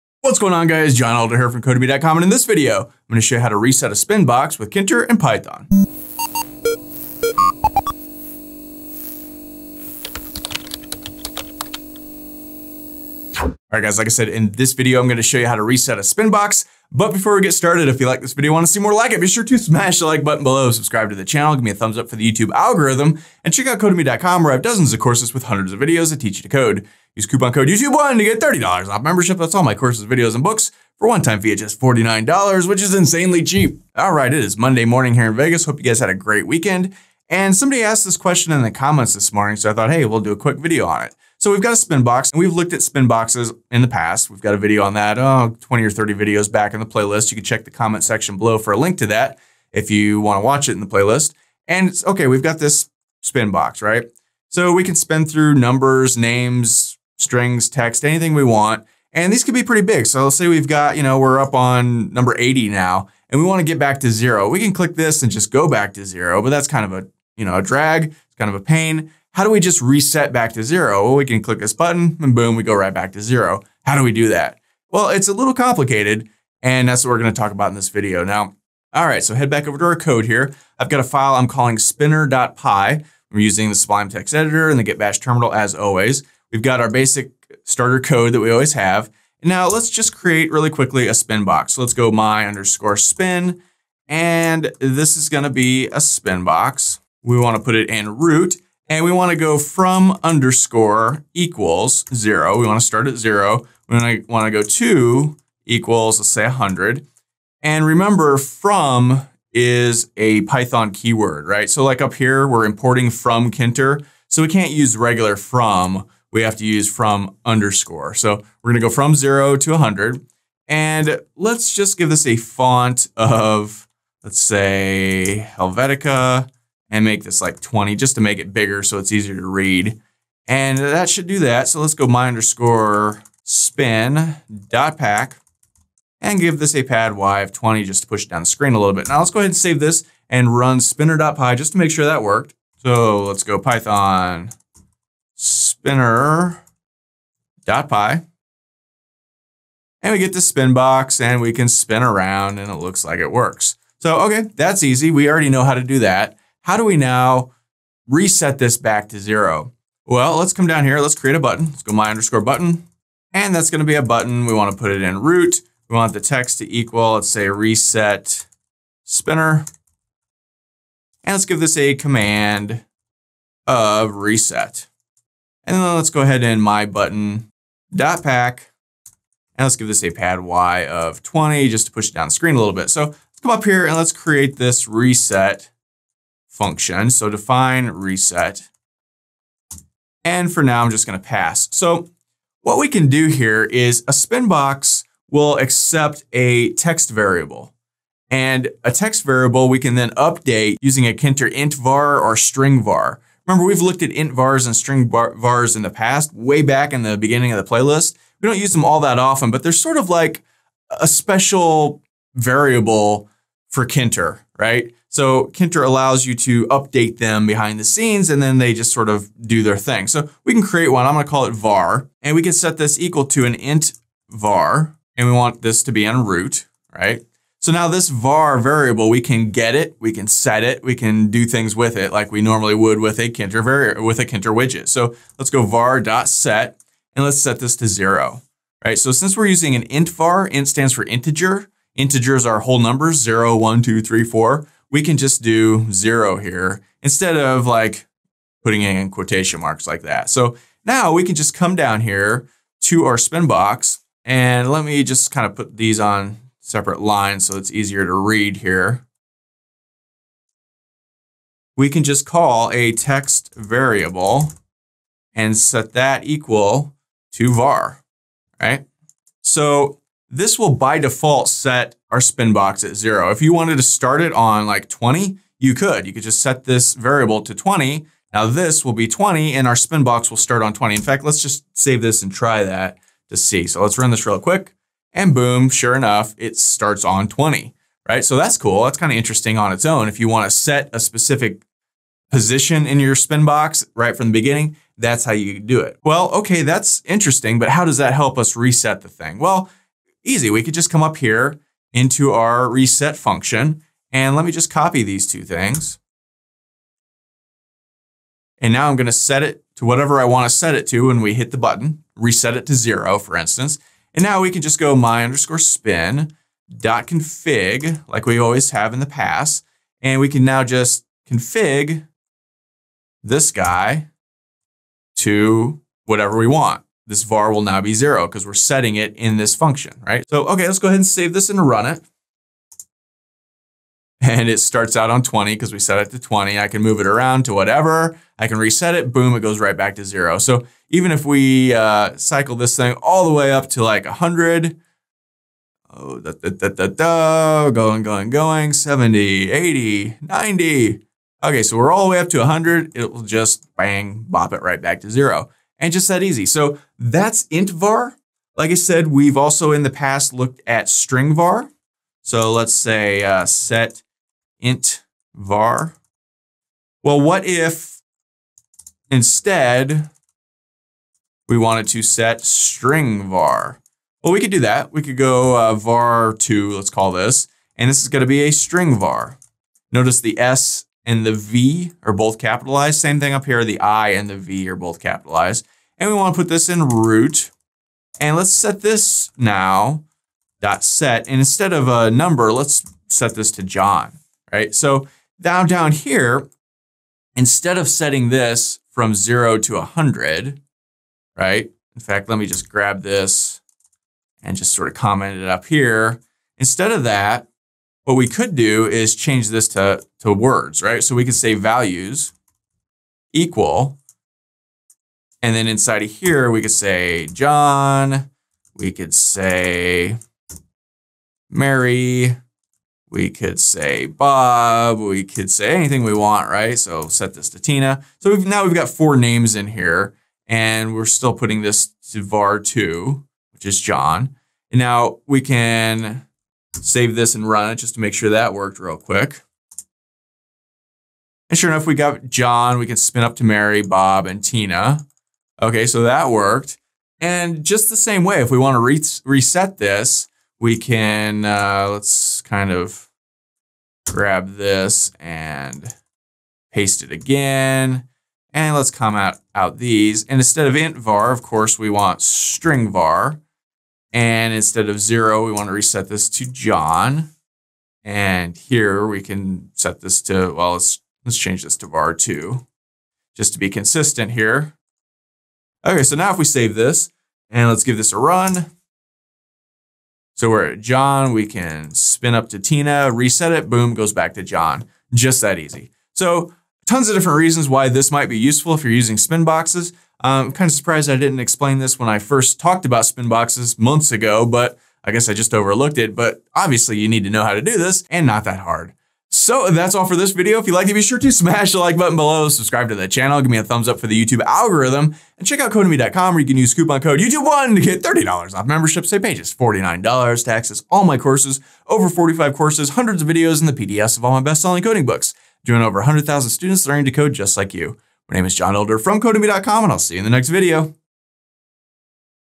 How To Reset A Spinbox With Tkinter - Python Tkinter GUI Tutorial #130
In this video I'll show you how to reset a spinbox back to it's default in Tkinter.

Say you have a spinbox that spins between 0 and 100 and you've spun it on up to 100, but want to reset it to zero. How do you do that?  You don't want to hold the spin down button till it spins all the way back down to zero!

Instead we want to click a button to reset the whole thing.  That's what I'll show you how to do in this video, for both numbers as well as text!